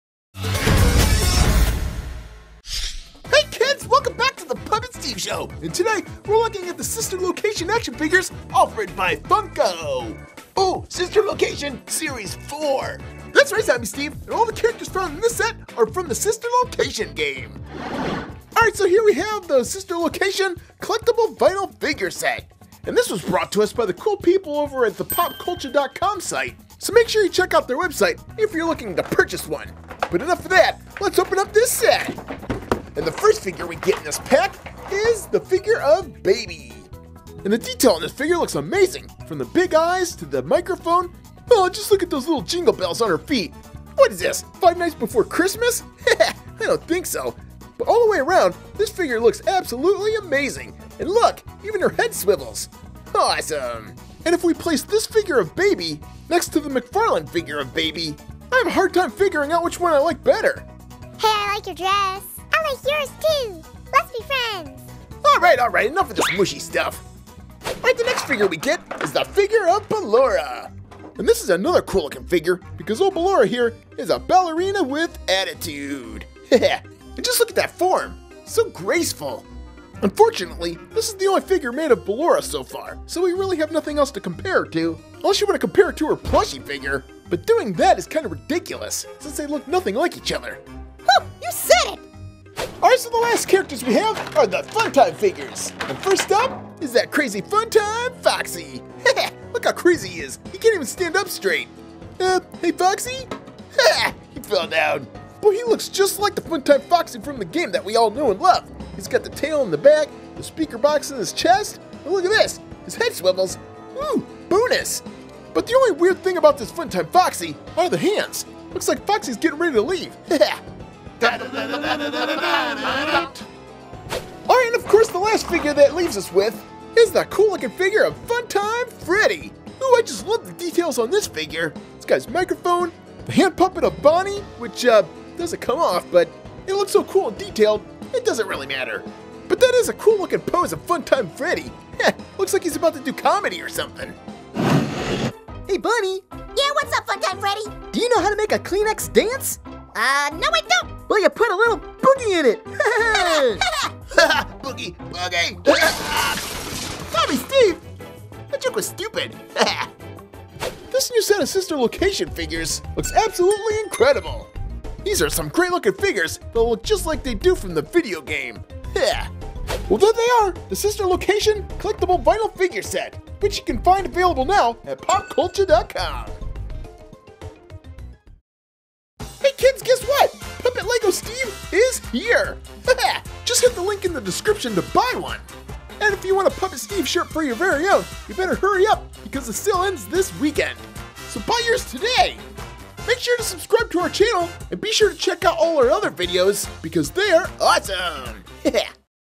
Five Nights at Freddy's FnaF Collectable FUNKO Sister Location SET 4 Vinyl Figures Mini unboxing
Five Nights at Freddy's FnaF Collectable FUNKO Sister Location SET 4 Vinyl Figures Mini unboxing Review

Puppet Steve Store (Buy Your Puppet Steve LEG0s and Spinners here!)